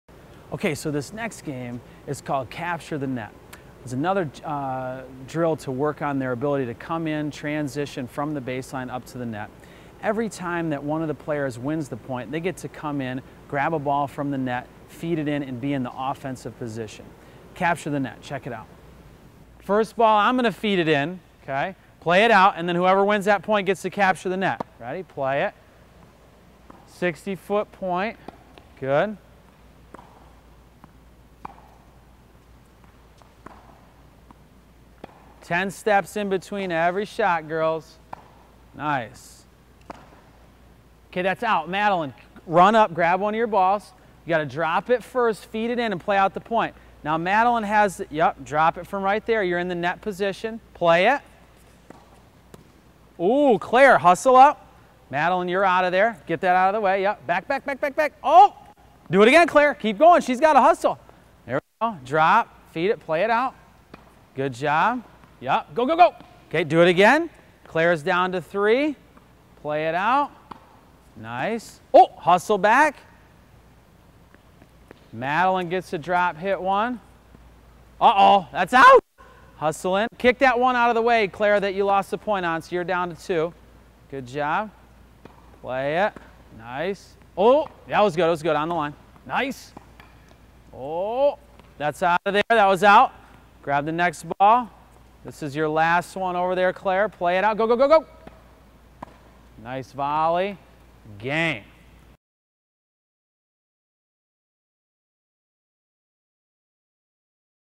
TennisDrills.tv (Little Kids 60 ft. Tennis Drills Sample)
Jorge Capestany and Dave Brouwer show you a Tennis Drills on a 60 foot court called Capture the Net. Check out more than 700 more Tennis Drills and Tennis Tips